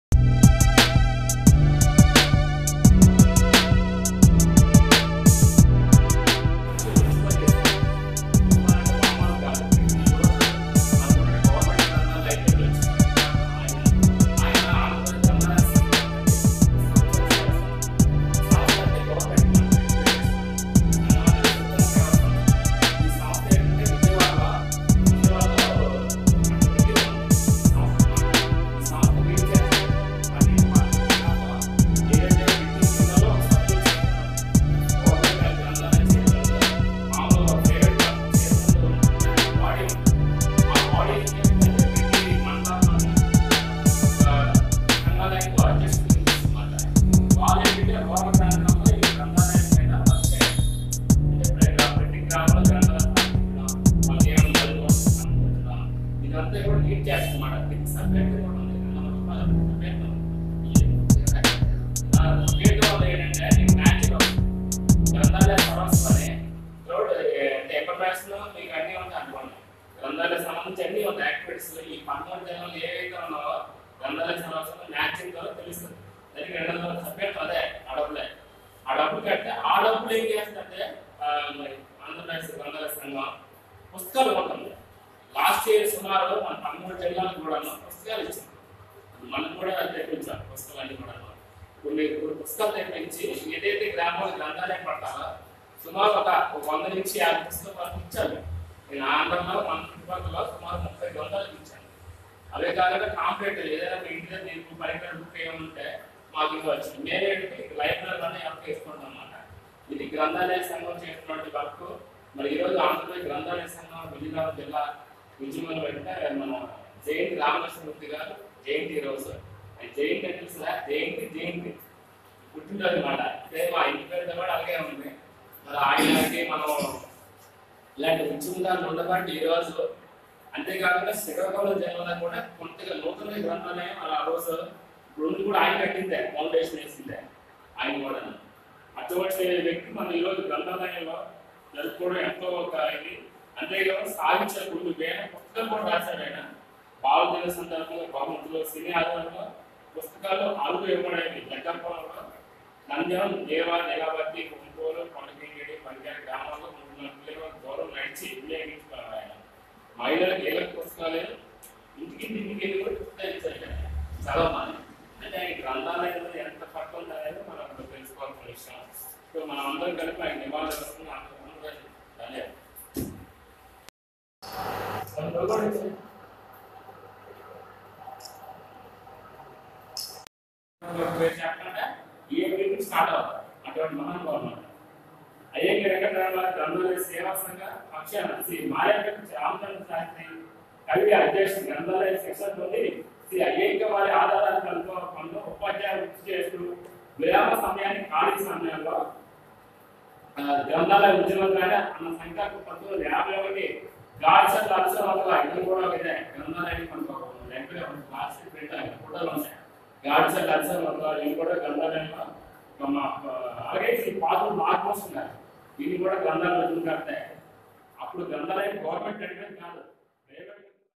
ASAYA Youth Association AYA
*ఘనంగా గ్రంధాలయోద్యమ పితామహుడు స్వర్గీయ జయంతి రామలక్షణ మూర్తి గారు 87 వ జయంతి ఉత్సవాలు నిర్వహణ*

*చీపురుపల్లి జి.అగ్రహారం ప్రజా గ్రంథాలయం,ఆశయ యూత్ అసోసియేషన్, మరియు ఆంధ్రప్రదేశ్ గ్రంథాలయ సంఘం జిల్లా గ్రంథాలయం  వారి ఆధ్వర్యంలో  నిర్వహణ*

తేదీ 1/07/2018 నుండి 3/07/2018 వరకు రాష్ట్ర వ్యాప్తంగా జరుగుతున్న నేపథ్యంలో తేదీ 1/07/2018 న  గ్రంధాలయం లో ఘనంగా నిర్వహించారు

ఈ సందర్భంలో జరిగిన కార్యక్రమంలో మండల  గ్రంధాలయ సంఘము అధ్యక్షుడు రెడ్డి రమణ. *జయంతి రామలక్షణమూర్తి* చిత్ర పటానికి పూలమాల వేసి ఘనంగా నివాళులు అర్పించారు ఈ సందర్భంగా మాట్లాడుతూ
విజయనగరం జిల్లా జగ్దపురంలో జన్మించారు అని 42 సంవత్సరాల ఉపాధ్యాయు వృత్తి చేపట్టుతు, గ్రంథాలయ ఉద్యమ లో పాల్గొన్నారు తన గ్రామంలో Sri రామ జయంతి గ్రంథాలయం స్థాపించి ఇంటి ఇంటికి తిరిగి పుస్తకాలు ఇచ్చేవారని  1946లో రాతి బడి నిర్వహించరని అన్నారు

కార్యక్రమంలో  ప్రజా గ్రంథాలయం సభ్యులు. త్రినాధ్,P. రాజేష్, చక్రి పాఠకులు పాల్గొన్నారు